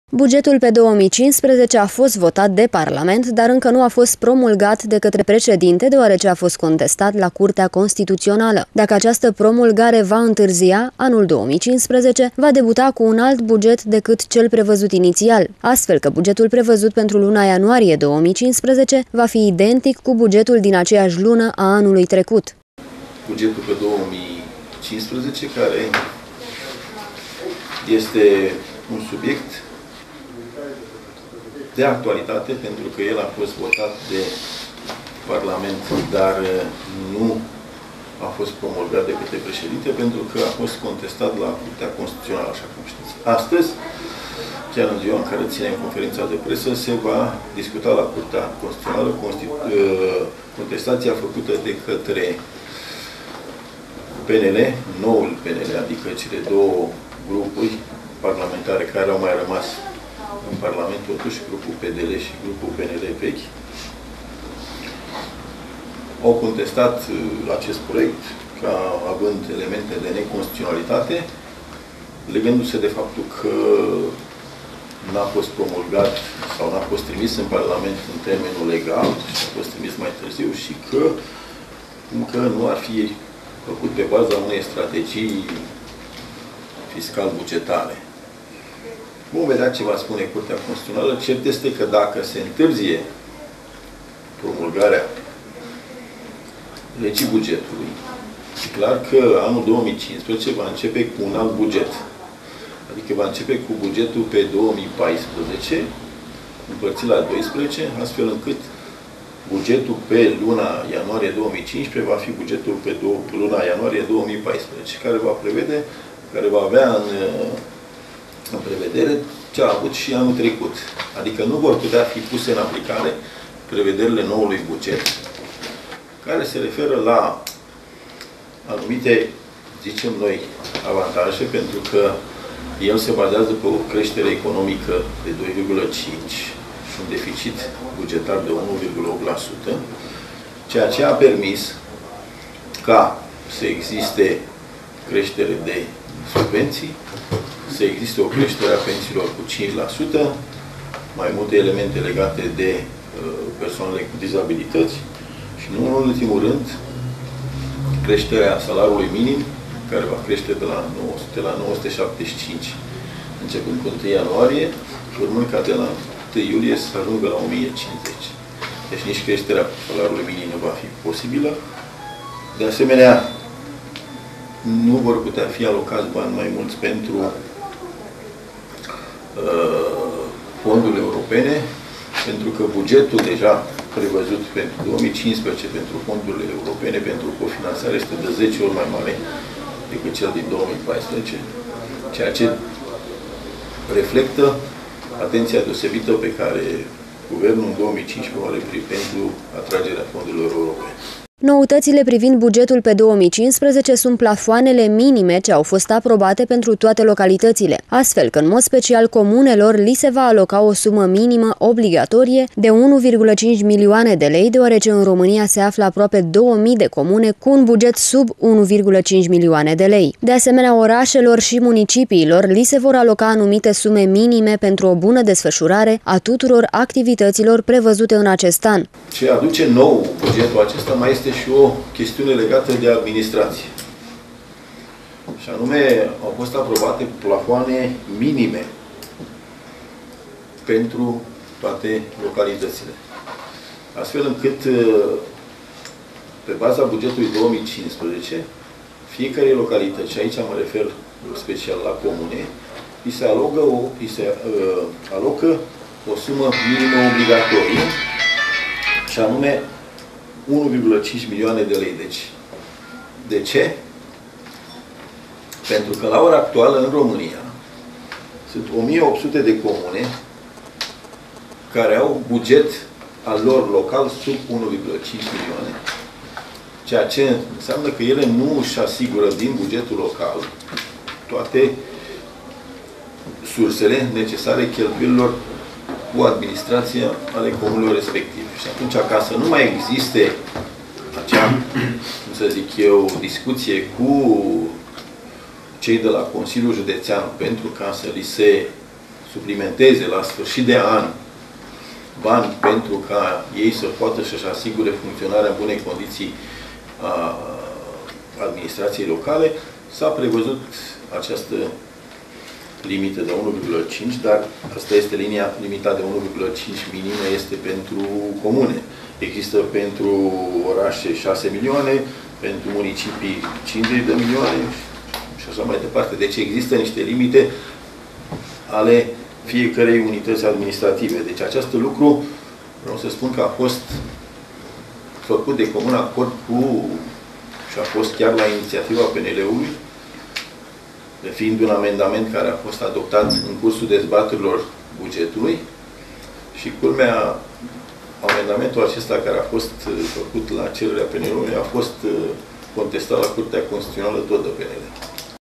stire 05 01 2015 conf psd buget
Cu numai trei zile înainte de încheierea anului, Organizaţia PSD Câmpulung, a organizat o conferinţă de presă, susţinută de deputatul Gheorghe Marin şi Liviu Ţâroiu. Pentru că s-a tras linie peste anul ce a trecut, au intrat în discuţie mai multe subiecte de interes local şi nu numai. Printre acestea, bugetul pe 2015 a fost primul subiect dezbătut în cadrul acestei conferinţe.